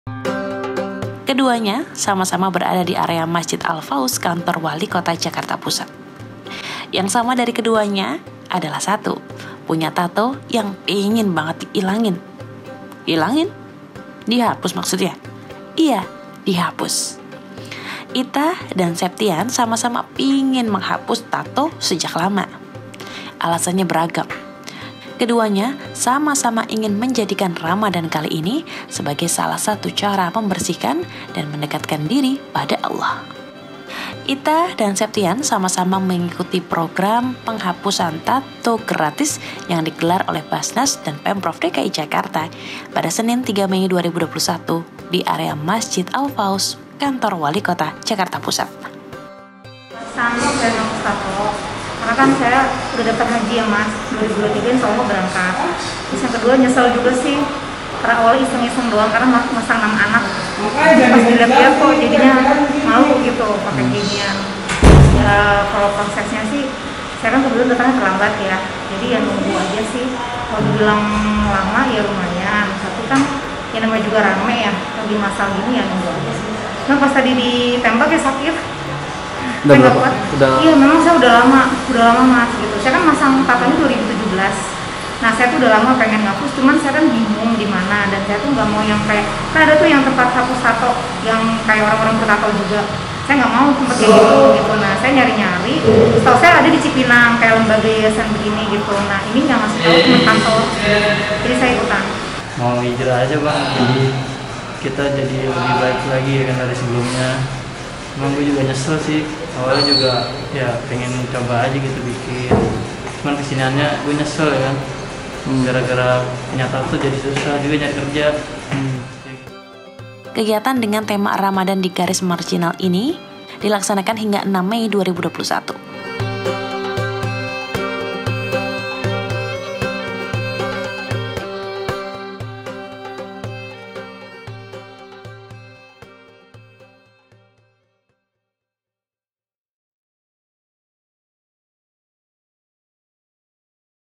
Cerita Haru Hapus Tato, Rasanya Menyesal Sampai Ubun-ubun
Baznas dan Pemprov DKI Jakarta menggelar hapus tato. Kabar bahagianya, gelaran ini diselenggarakan secara gratis. Program ini digelar hingga 5 Mei 2021.

JANGAN LUPA INSTAL APLIKASI GENPI.CO :
APPSTORE: